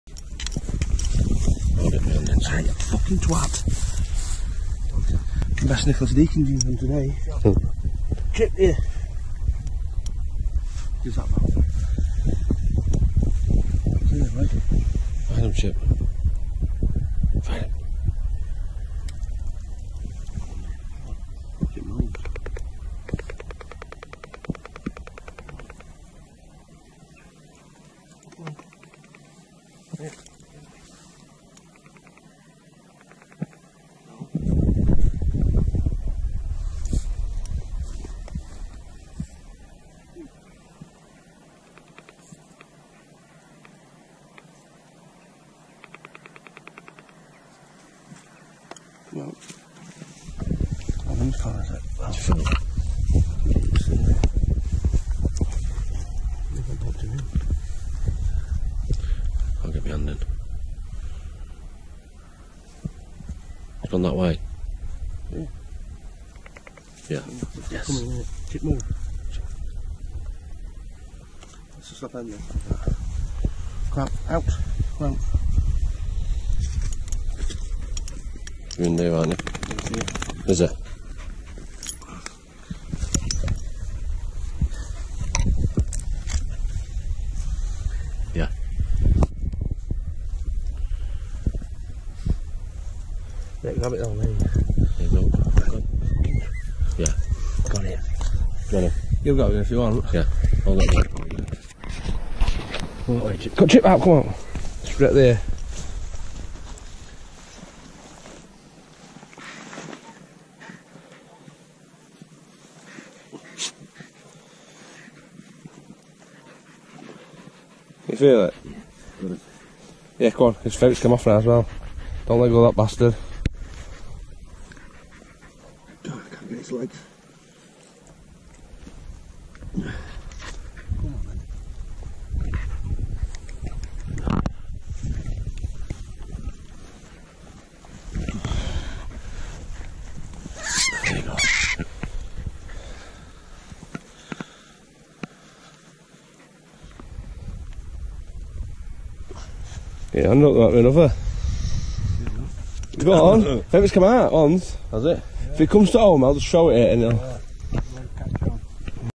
Ferreting, locating, digging for rabbits
First time out with the ferrets this season